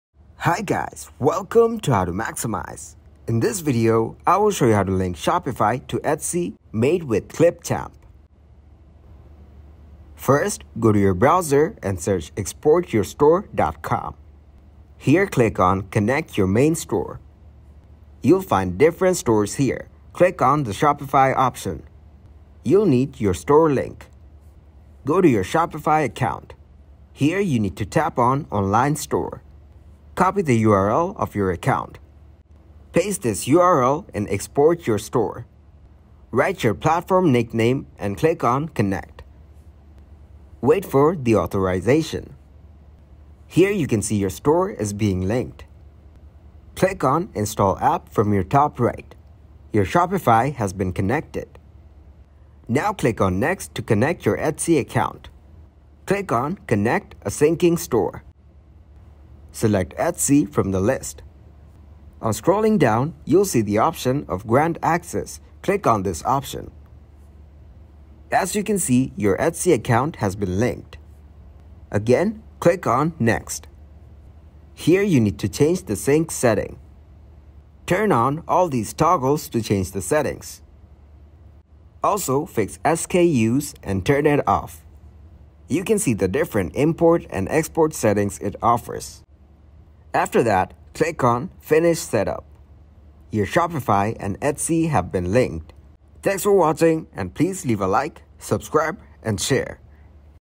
How To Link Shopify To Etsy Made with Clipchamp (UPDATED 2023)
In this tutorial, you will learn How To Link Shopify To Etsy  Made with Clipchamp in easy steps by following this super helpful tutorial to get a solution to your problem!!